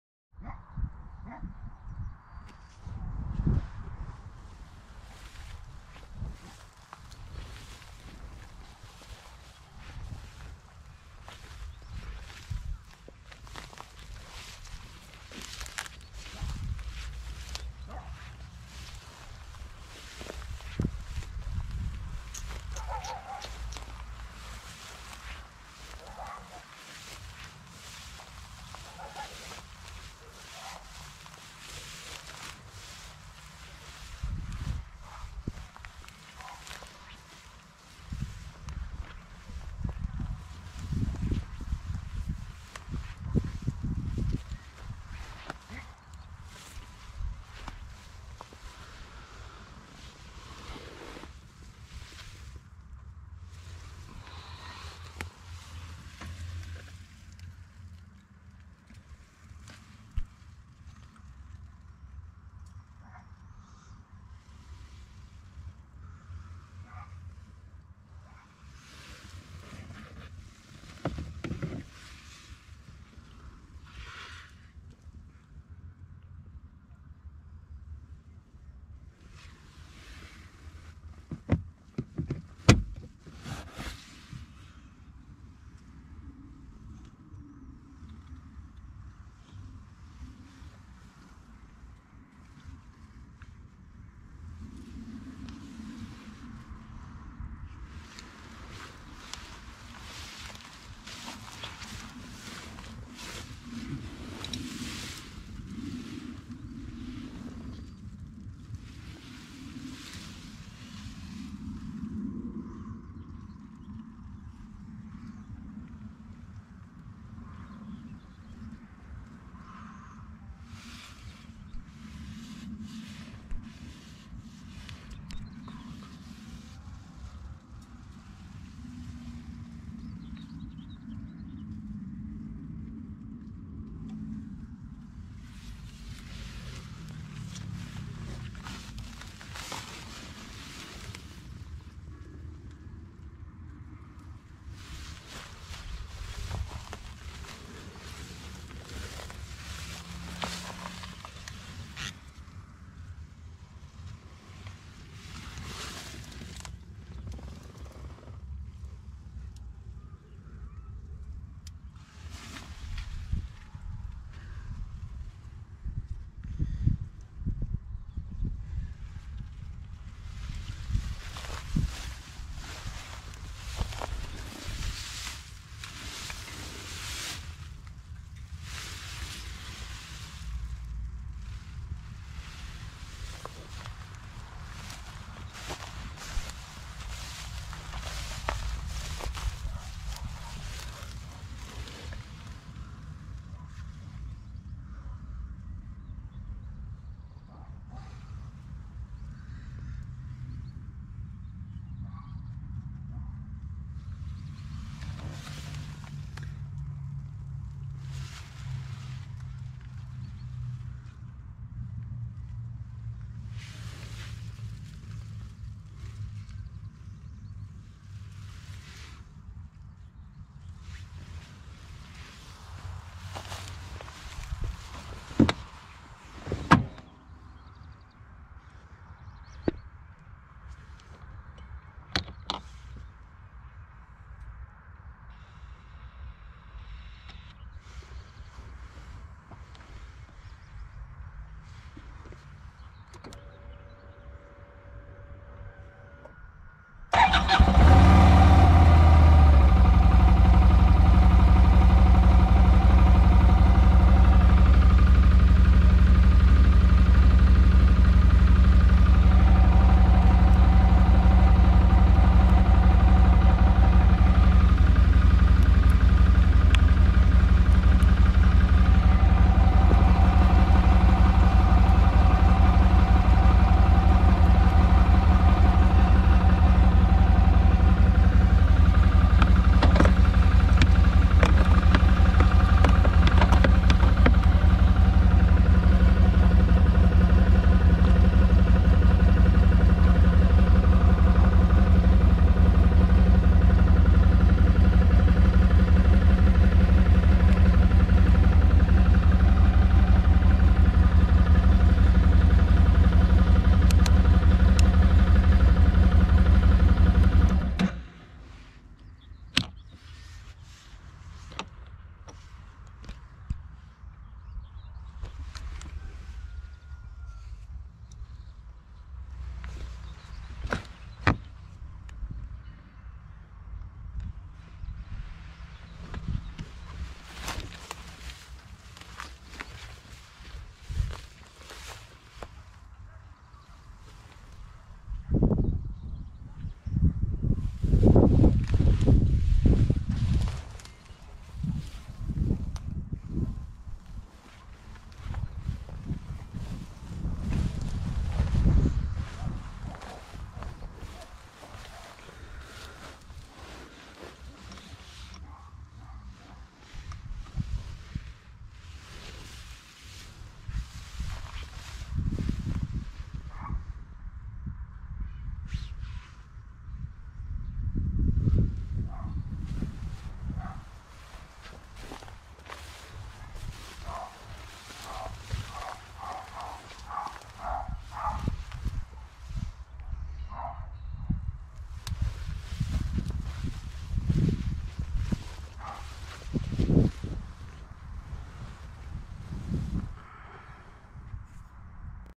Квадроцикл Odes 650 S реплика Can-Am Brp Outlander 650
Компания Odes не стали идти по пути создания полностью нового квадроцикла, а выбрали бузу хорошо зарекомендовавшего себя Brp Outlander 650 в короткой базе. Повторили основные узлы, мотор. коробку, передний редуктор остался классического типа с возможностью блокировки дифференциала.
Я так предполагаю скоро эта новинка выйдет под брендом Русская Механика.

Берегите себя